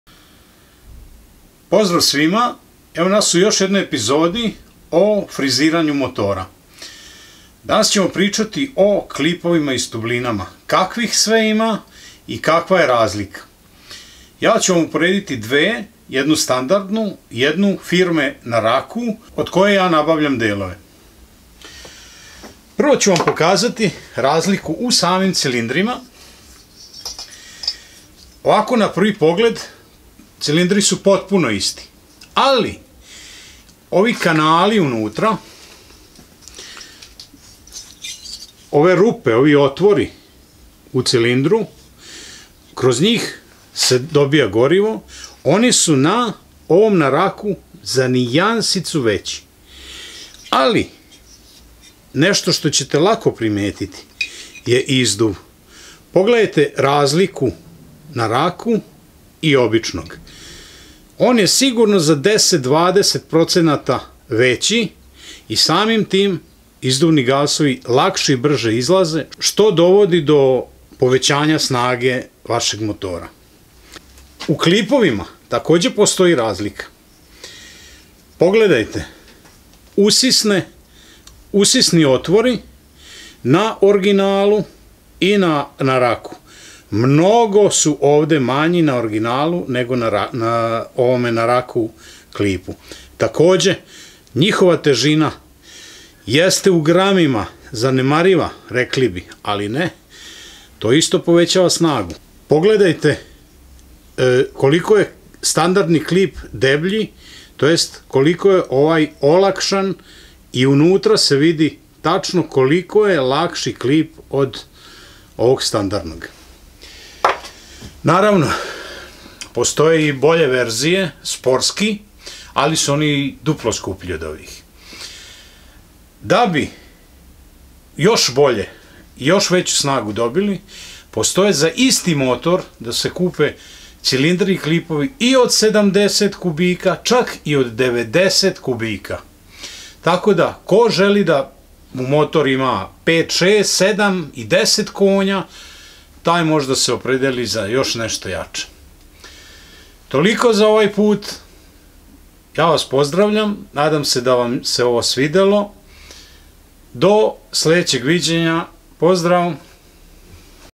Friziranje motora 3. deo (klip i cilindar)
U trecem delu o friziranju motora pricamo o klipovima i cilindrima.
Klip i cilindar su osnovni delovi motora i od njih zavisi koliko cemo snage dobiti. Kakva je razlika medju klipovima a kakva medju cilindrima koja se lako moze uociti mozete videti u sledecm video klipu.